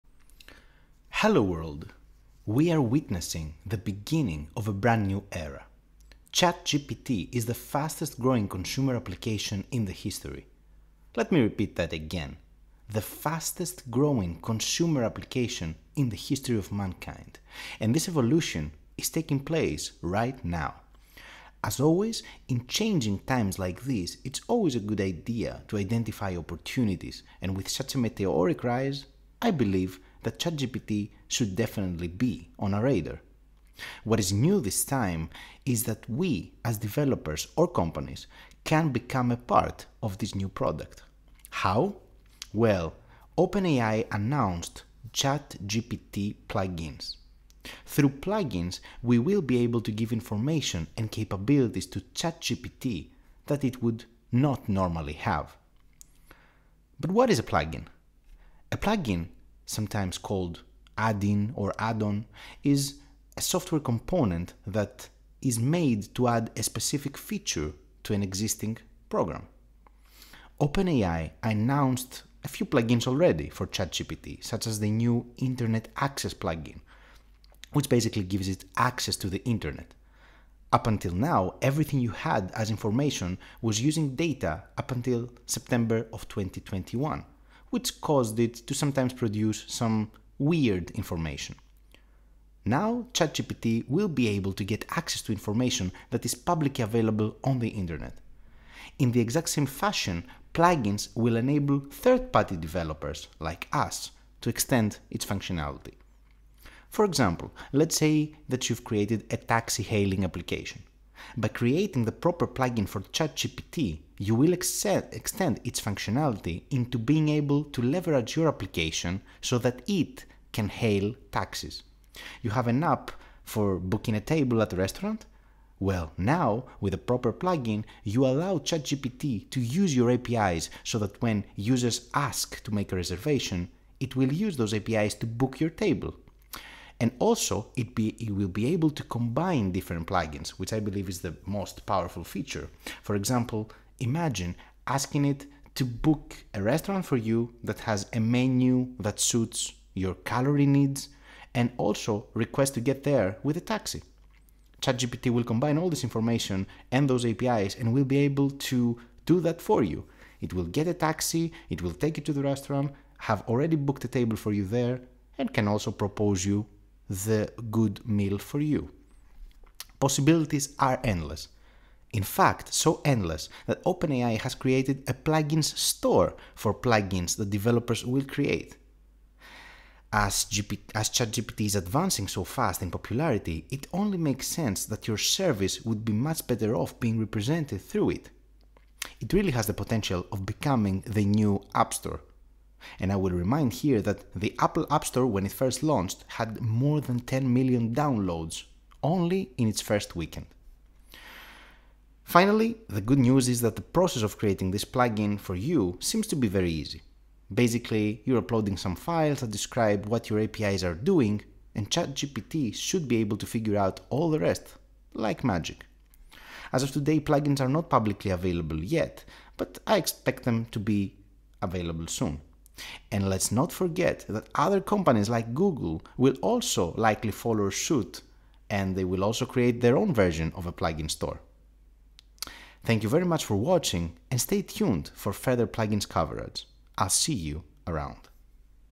ChatGPT Plugins Store is the new App Store
Hello World, ChatGPT Plugins are here, and it's beneficial to be aware about it.